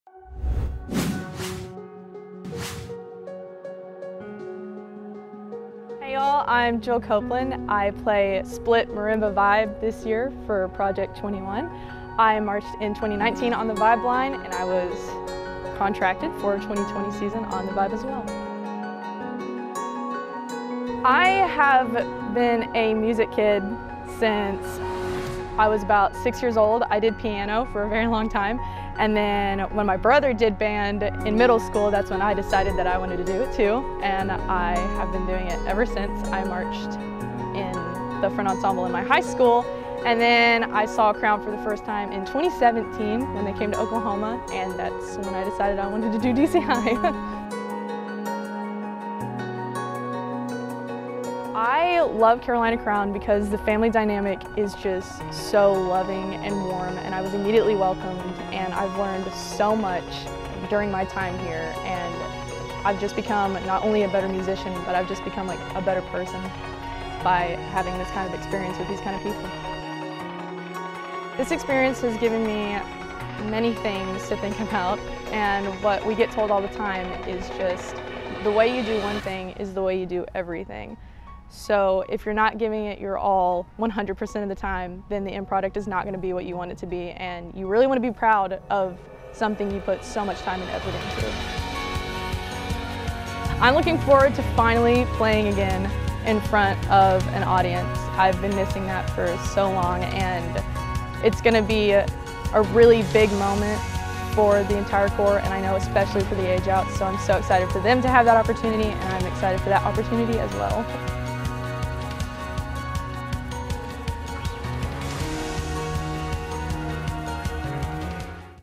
Member Spotlight | Jill Copeland
— 𝗠𝗘𝗠𝗕𝗘𝗥 𝗦𝗣𝗢𝗧𝗟𝗜𝗚𝗛𝗧 —
Meet Jill Copeland from Owasso, Oklahoma! Jill plays split marimba-vibe in the Front Ensemble and has been a member of Carolina Crown since 2019.

📲Follow us on all Crown media pages for the latest content!